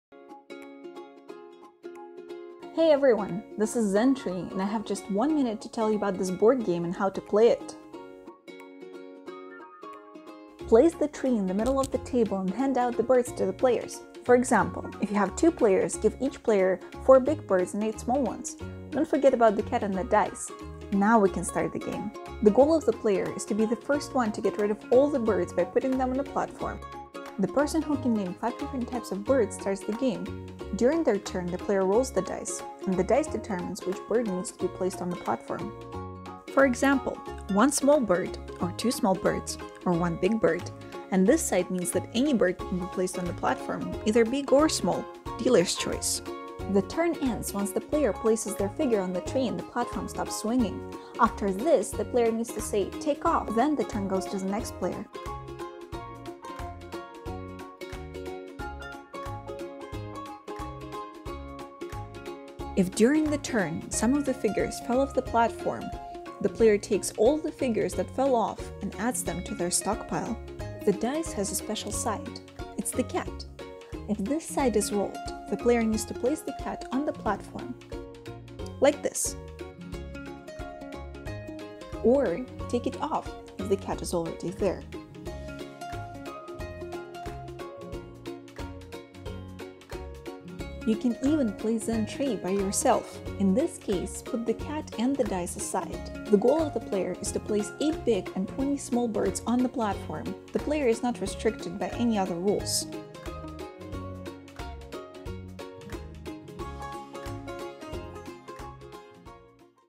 Board game «Zen Tree». Game rules.
ZEN TREE
Age 3+
Time 20 minutes
Players 1-4

The players take turns to draw a card from the pack and open then. There is a picture of a Toddle-Bobble on the card. If this Toddle-Bobble appears for the first time, the player who opened the card gives him a name. Everyone needs to remember this name. The player who remembers the name correctly and is the first to shout it out next time when the toddle-bobble comes up in the game, will claim all the open cards to his winning pile.

Attention! The game activates superpowers:
quick responce, concentration, lots of fun